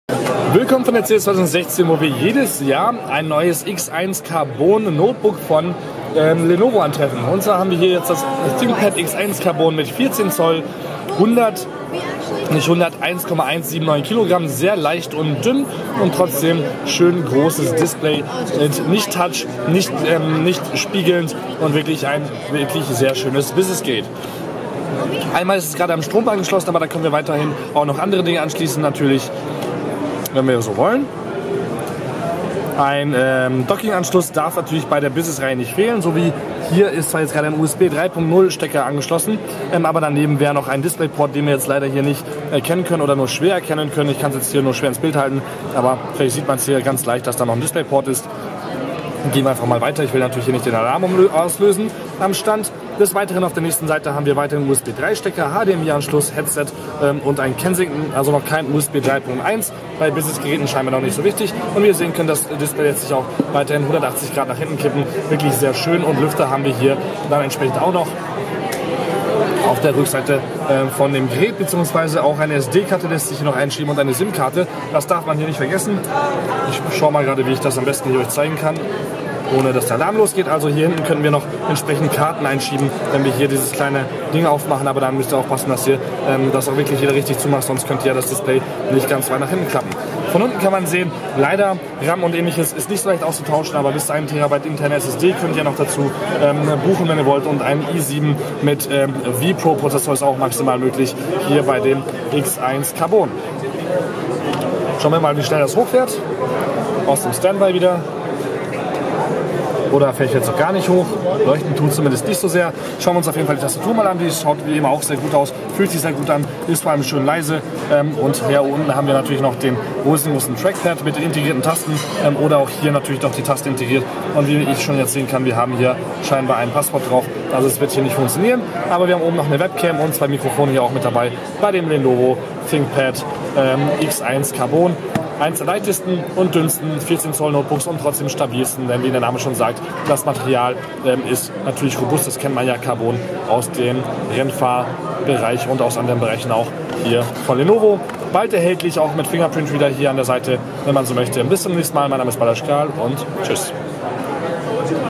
Lenovo Thinkpad X1 Carbon 2016 erster Eindruck [4K UHD]
Lenovo Thinkpad X1 Carbon kurz in Las Vegas auf der CES 2016 angeschaut. Wieder die neuste Hardware drin und weiterhin super verarbeitet. Uns gefällt das schicke Notebook weiterhin.

folgen!!!

Meinen Flaschenöffner Ring gibt es hier für 3 Euro mit 2 Stück in der Packung zu kaufen